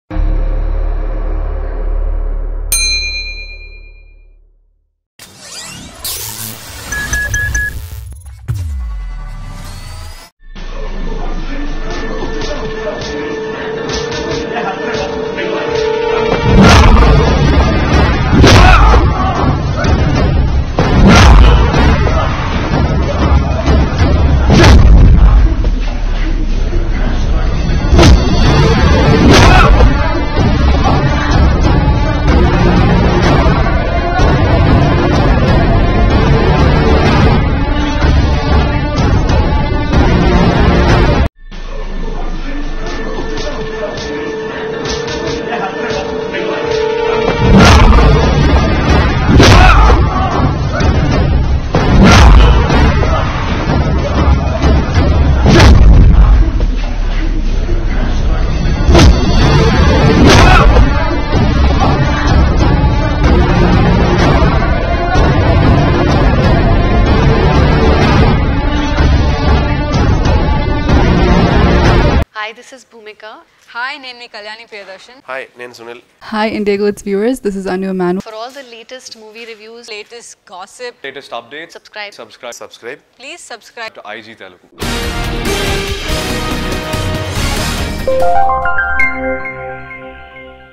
లాగి కొట్టిన వర్మ - మామూలుగా కాదు || Stunts of RGV - The fighter || Ram Gopal Varma
GST director Ram Gopal Varma aka RGV's real life stunts.